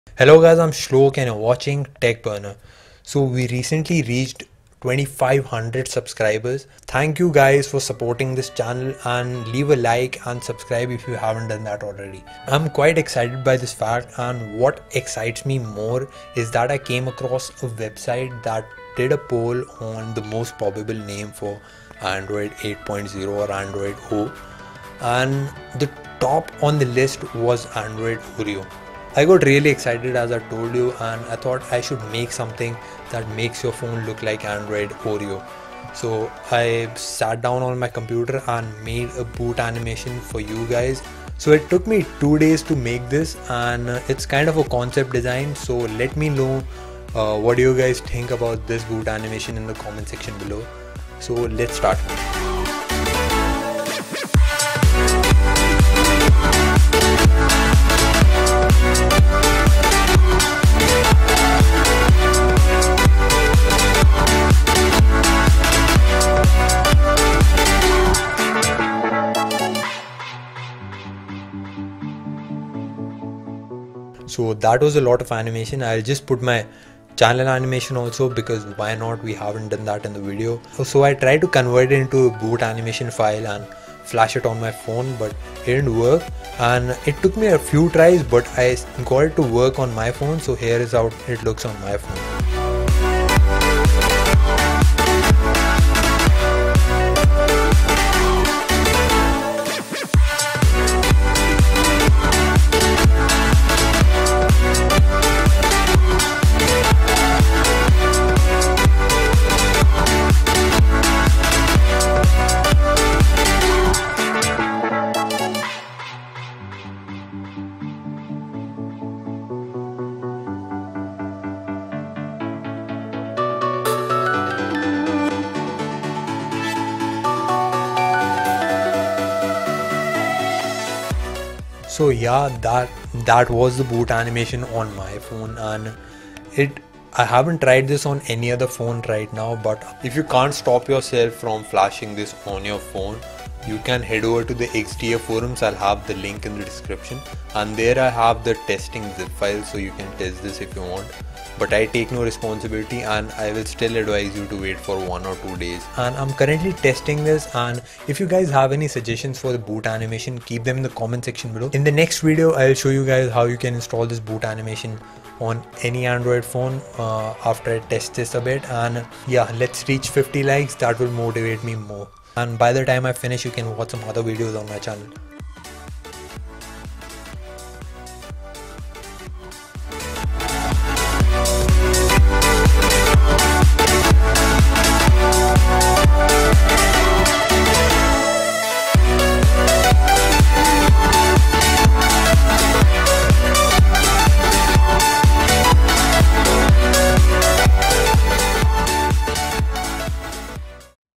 Android Oreo 8.0 Boot Animation Concept
This video has the Android Oreo concept boot animation . I was very excited because i love oreo and i wanted to make this bootanimation for you guys!

Please tell me how you feel about the bootanimation in the comments below!

This REQUIRES ROOT!

TESTING IS OVER!!!

All the downloads are in the new video and also below

bootanimation.zip

Let me know if i forgot anything!

More Stuff!

Hopefully you liked the video ! Enjoy